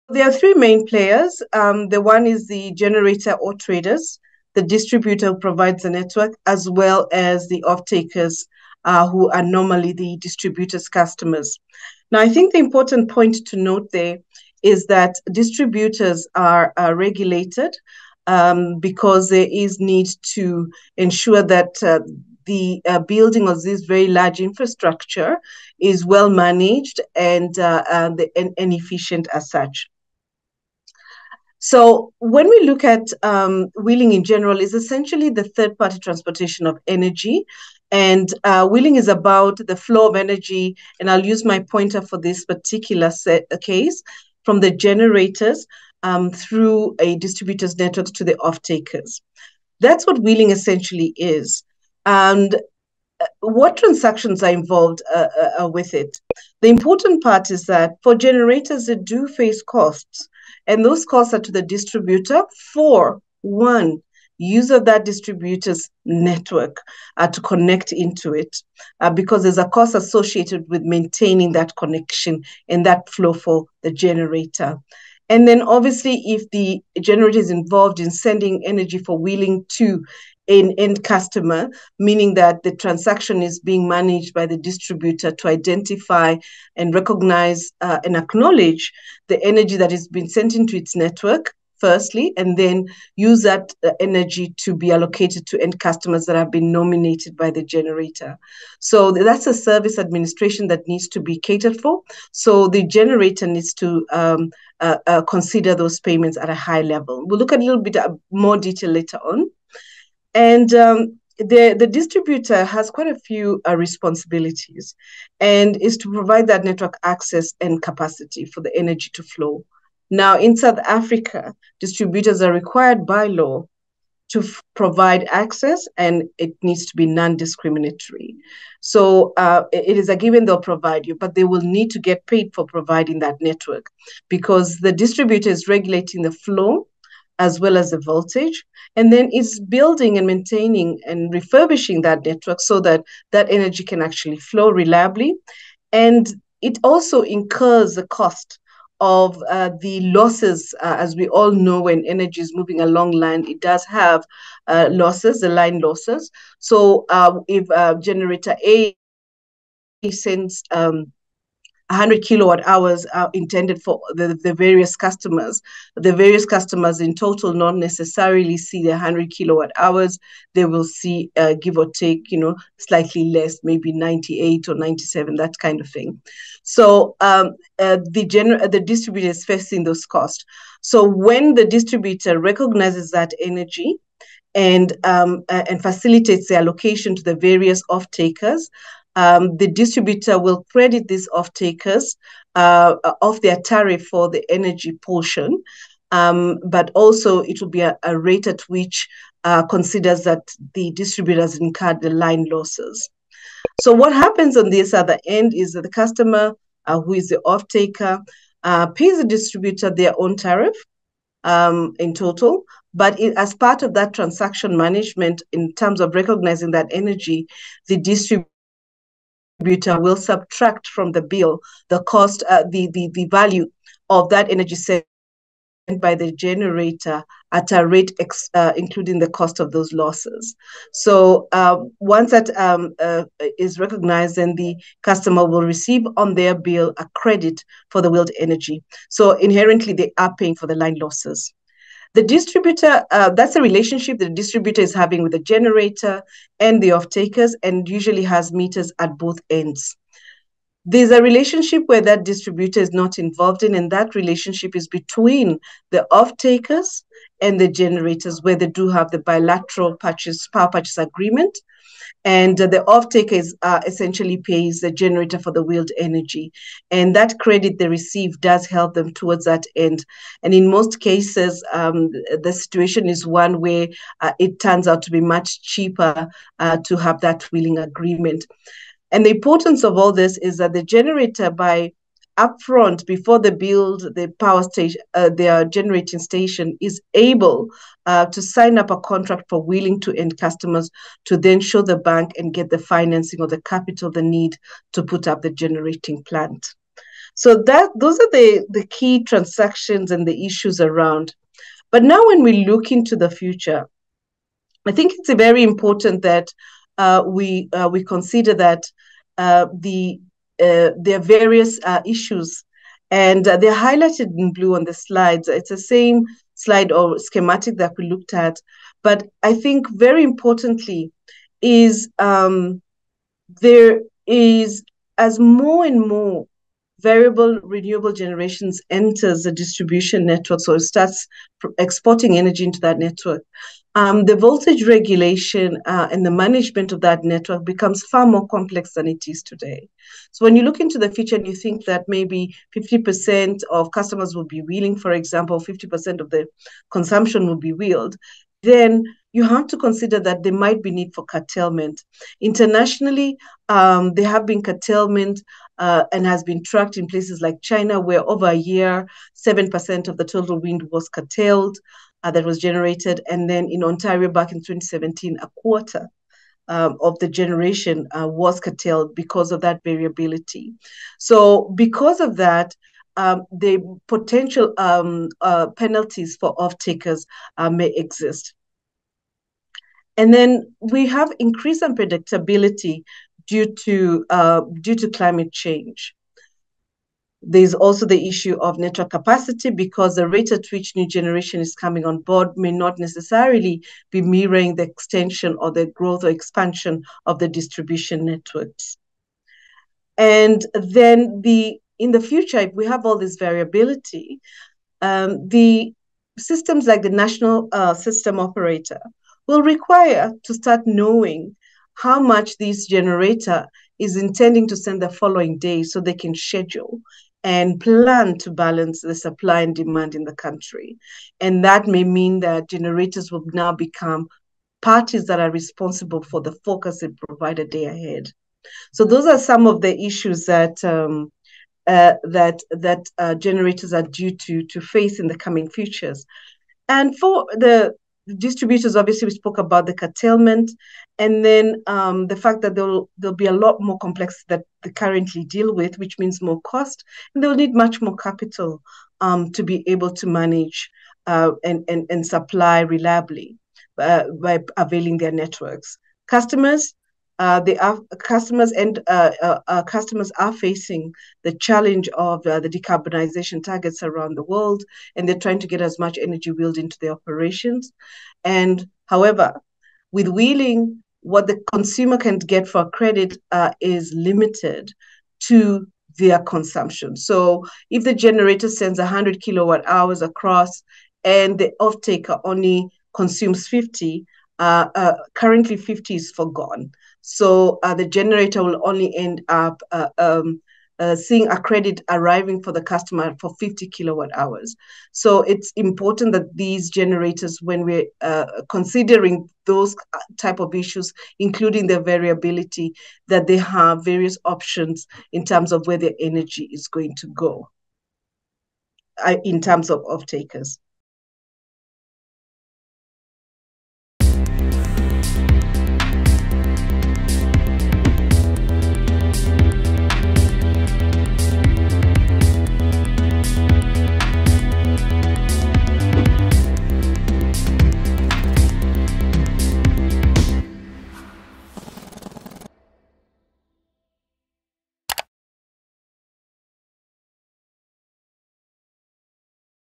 Speakers outline considerations for unbundled tariffs, wheeling framework
As the unbundling of State-owned utility Eskom gains momentum, both internally and through legislative processes, the unbundling of tariffs is equally important, and there are number of considerations for this, speakers averred during the South African Independent Power Producers Association’s ‘Tariff and Wheeling’ webinar on November 2.